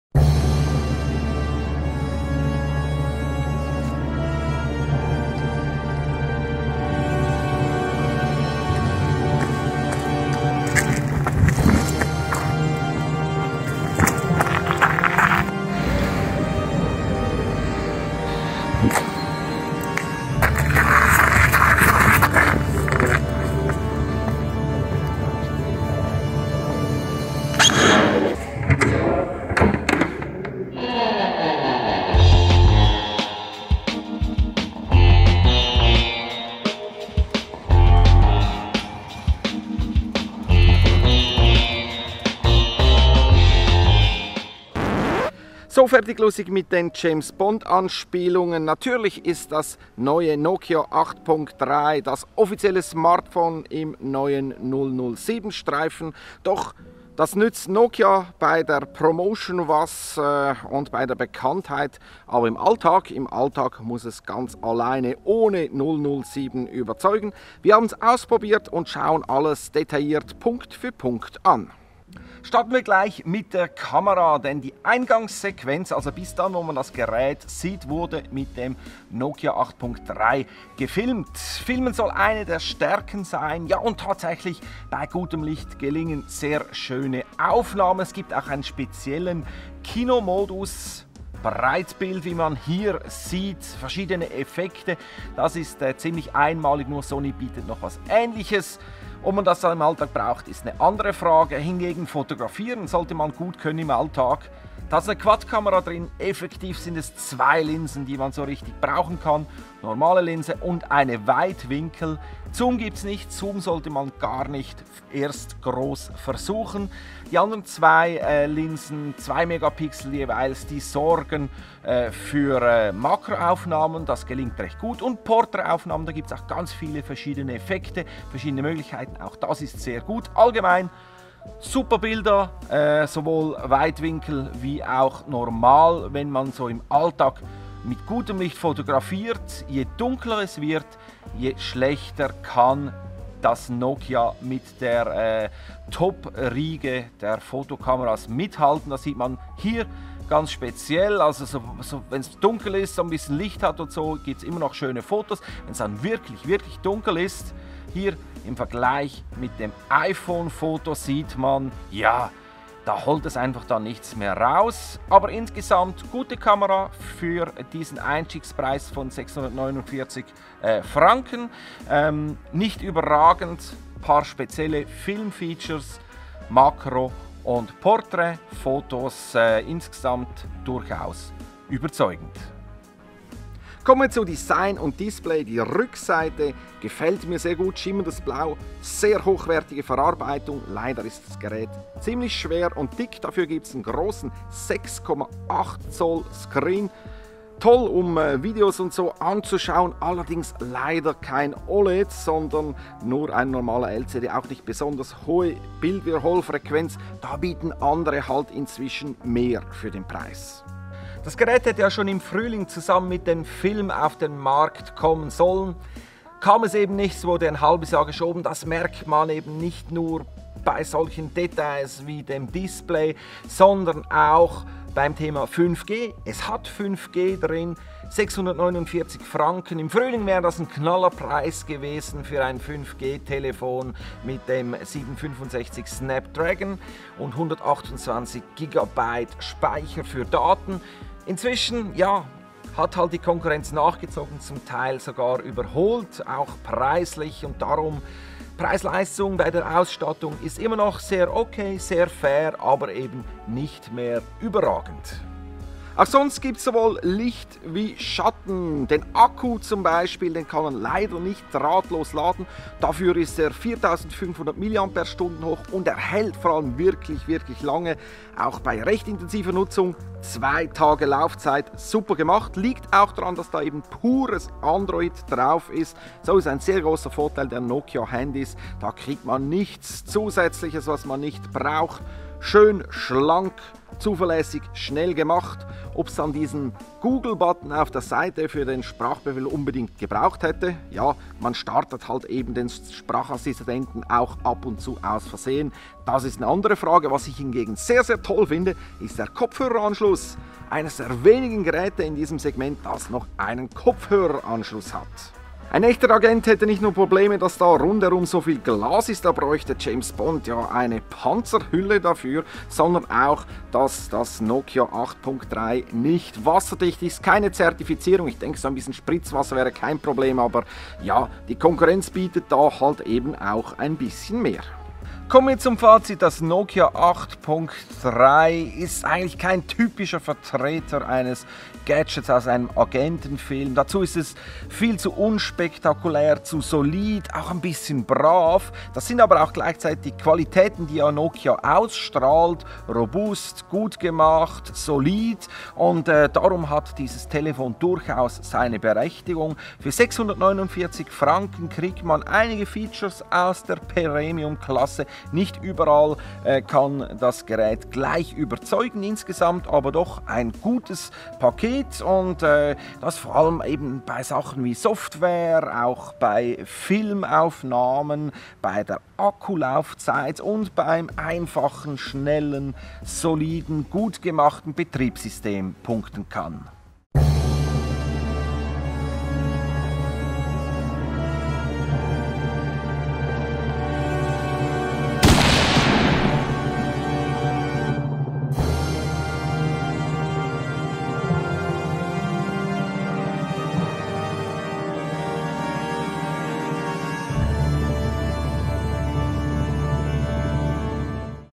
Nokia 8.3 Review / Test: Die WAHRHEIT über das James-Bond-Smartphone von Nokia (deutsch)
Mehr Tests, News, Unboxing und Vergleich auf dem neuen Schweizer Gadgetportal

Nokia ist der offizielle Ausrüster von 007 im neuen James-Bond-Film «No Time to Die». Und wie der Film selber kommt auch das Nokia 8.3 5G mit einem halben Jahr Verspätung.
Das neue Smartphone der finnischen Marke hat den Anspruch, in der oberen Mittelklasse mitzuspielen – und bietet zu einem Preis von 649 Franken viele Features.
Doch wie gut ist das 6,8-Zoll-Gerät wirklich? Wie bewährt sich das Agenten-Phone im profanen Alltag? Der ausführliche Videotest zeigt es. Und natürlich darf auch ein bisschen James-Bond-Atmosphäre nicht fehlen.

Hat das Video gefallen? Über Kritik und Diskussionen in den Kommentaren freue ich mich – genauso wie über Abos für diesen YouTube-Kanal.